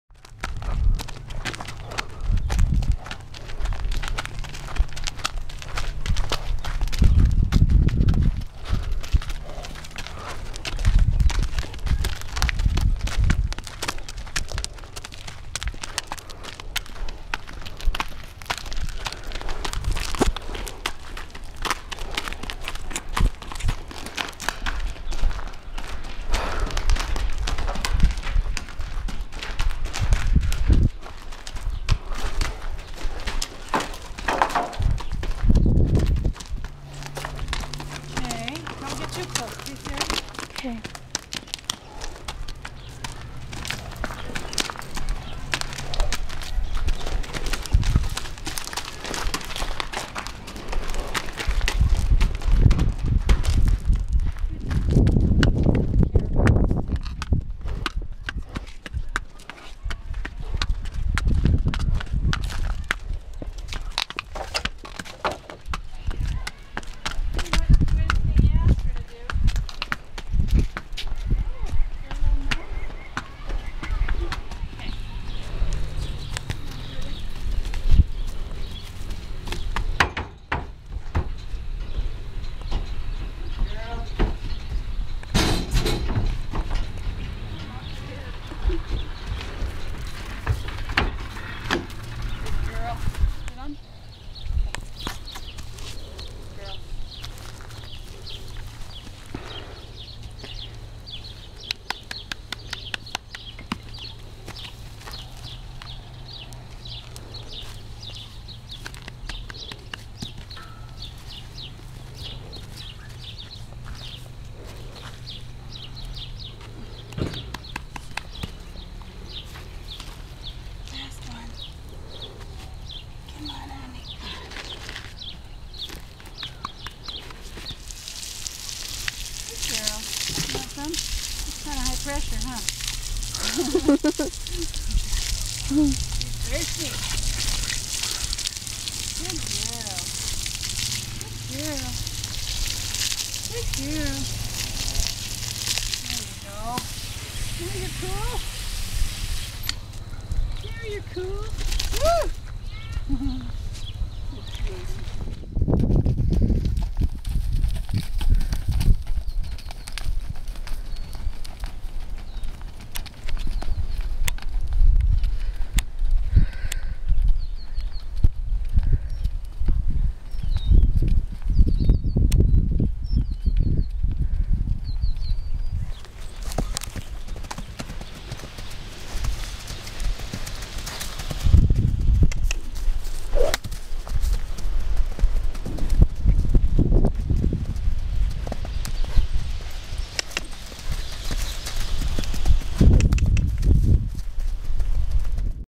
Rare Dun Fox Trotter Filly!
Rare dunlino Fox Trotter Filly.  Super well gaited and will be a dream ride when grown and trained.  She will be 2 in October.  Very smart and willing to please.  Has been worked with extensively and is a pleasure to have at the farm.  She loves the company of people!!  I am 5' 8" and 170 lbs. so you can get an idea of her current size.  For sale $2,800.   SOLD